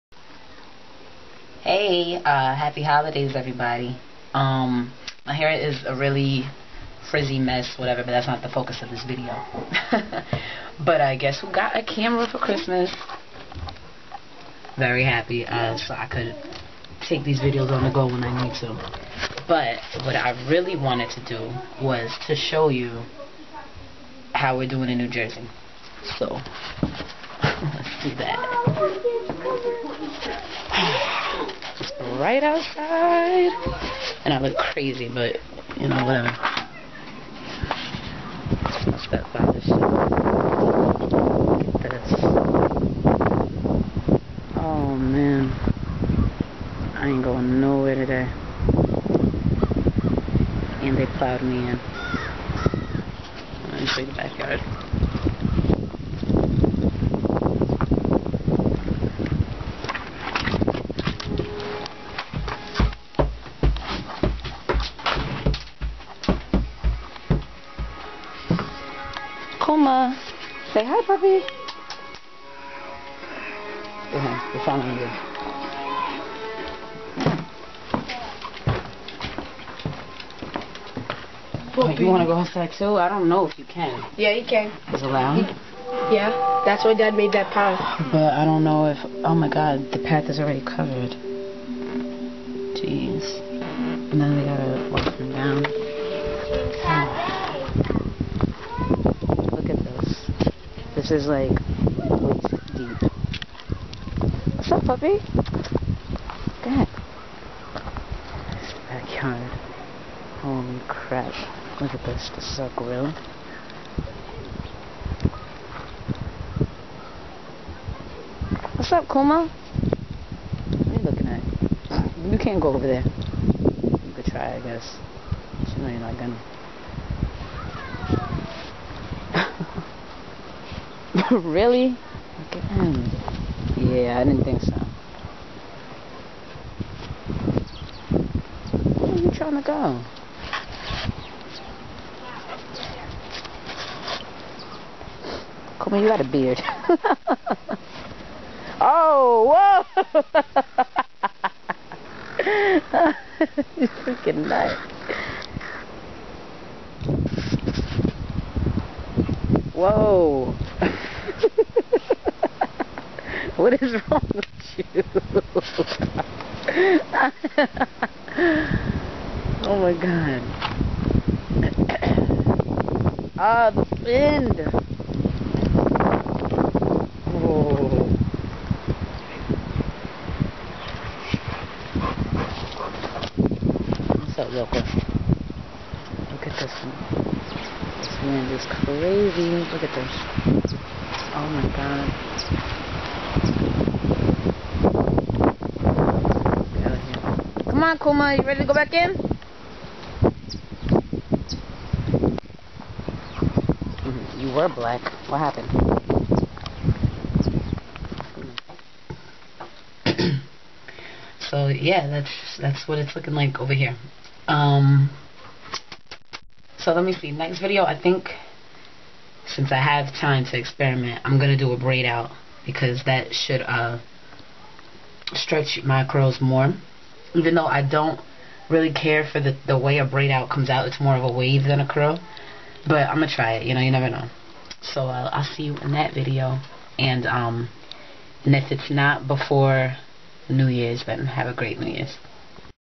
Happy Holidays from me n the Puppy!!
snow, snow, snow. more snow, some crazy natural hair, my dog, more snow lol